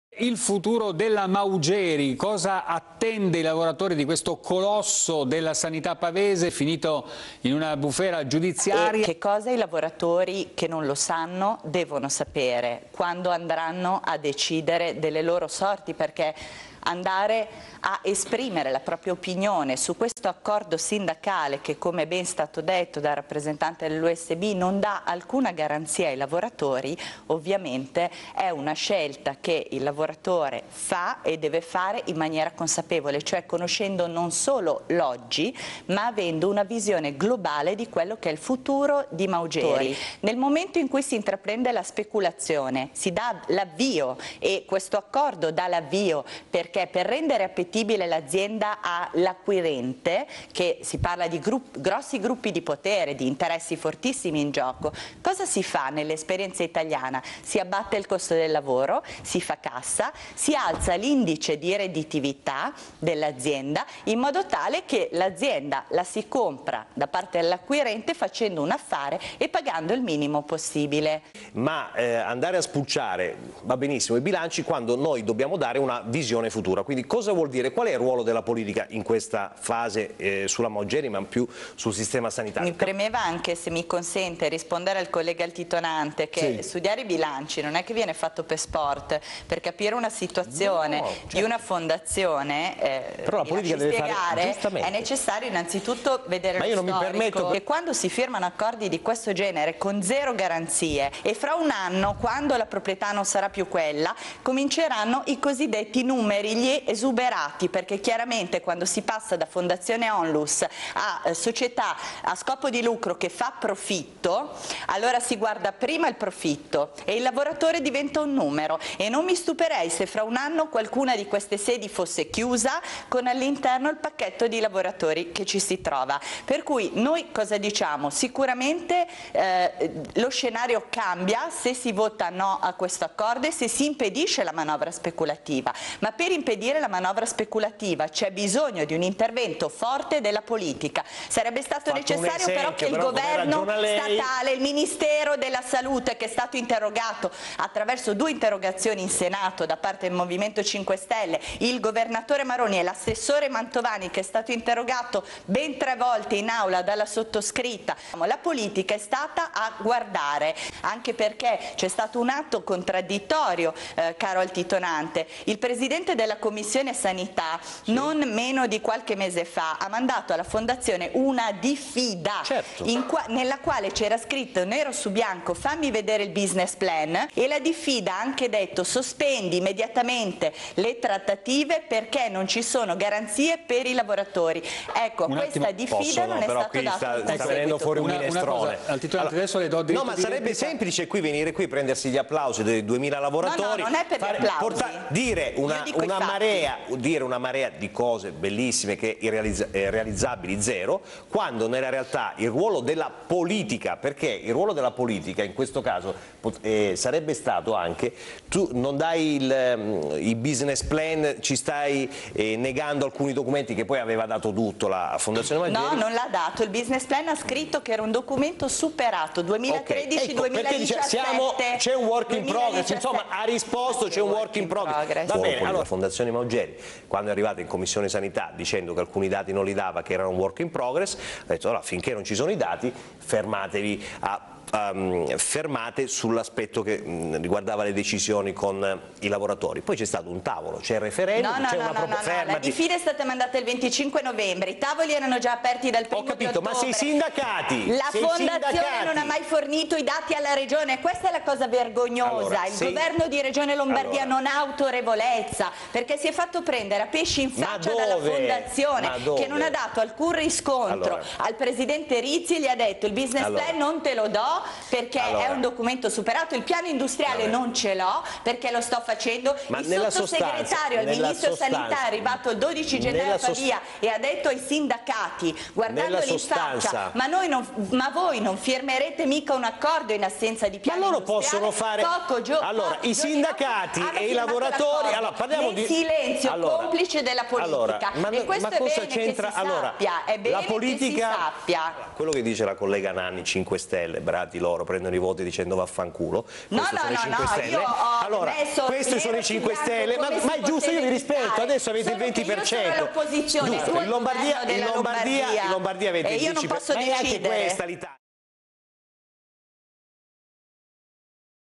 Fondazione Maugeri: tutta la verità! (Iolanda Nanni a TelePavia 30/01/2015)
Tutta al battaglia del M5S condotta nell'istituzione regionale per salvare i lavoratori della Fondazione Maugeri: